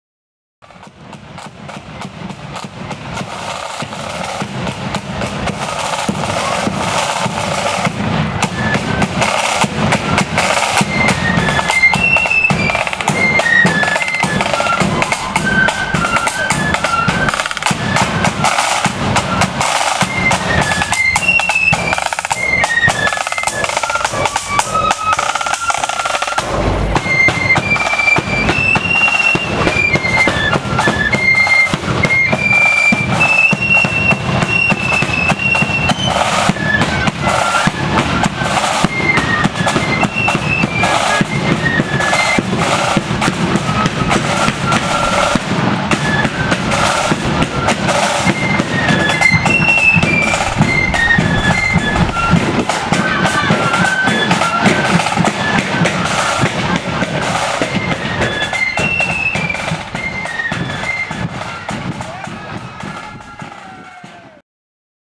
Ulster Grenadiers @ Rathcoole Protestant Boys Annual Parade
Ulster Grenadiers On the March at Rathcoole Protestant Boys Annual Parade on Saturday 25 June 2017